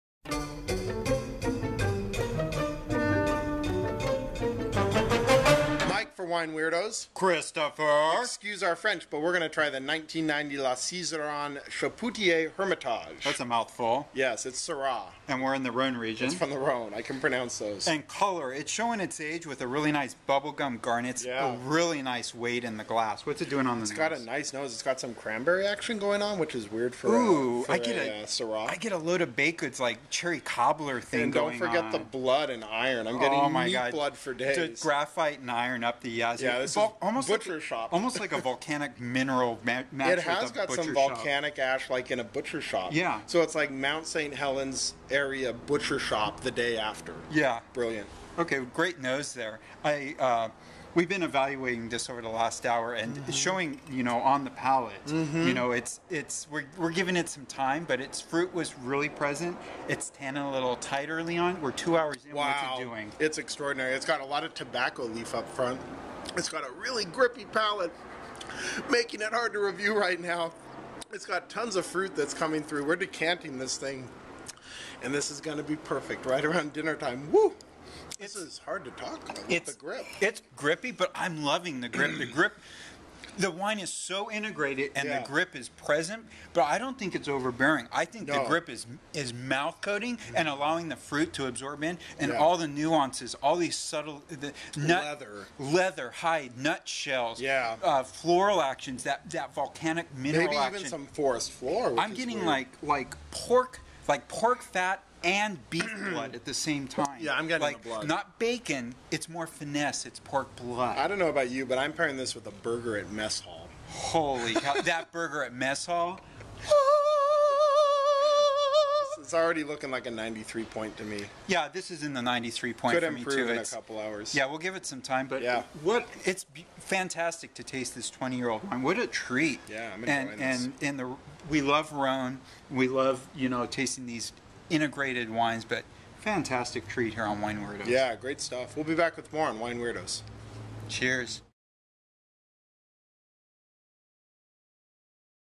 1990 M. Chapoutier Hermitage La Sizeranne, Aged and Complex Red Wine From The Northern Rhône Valley
M. Chapoutier
1990 Hermitage La Sizeranne
$110.00
France -- Rhône